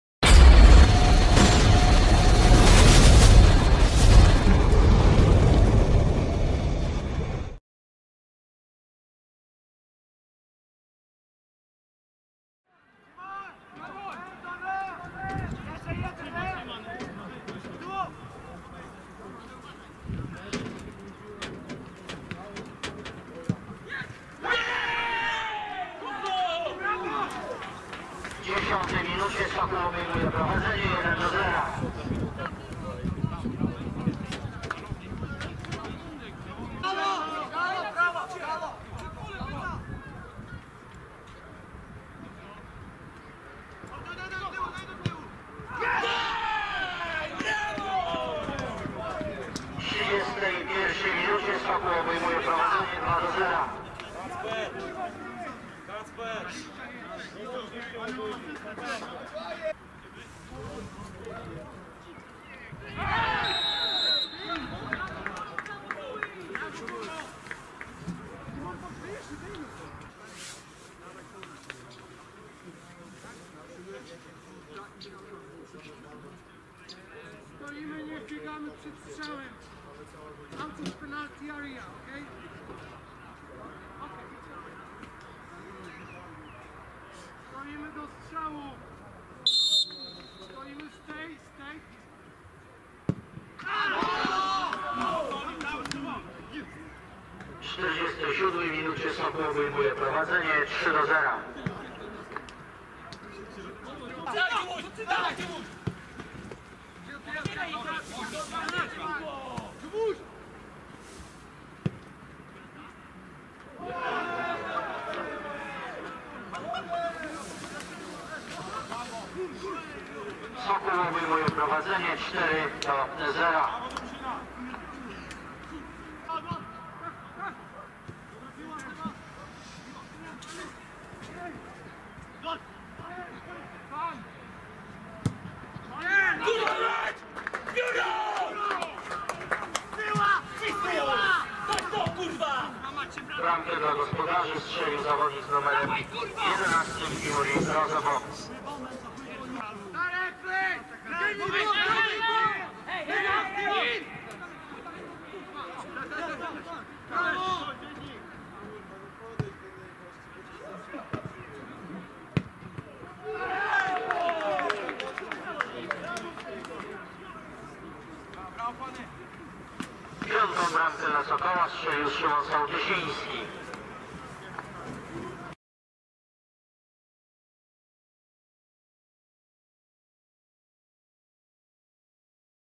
Bramki z meczu Kaczkan Huragan Morąg-Sokół Aleksandrów Łódzki
Mecz 22 kolejki 3 ligi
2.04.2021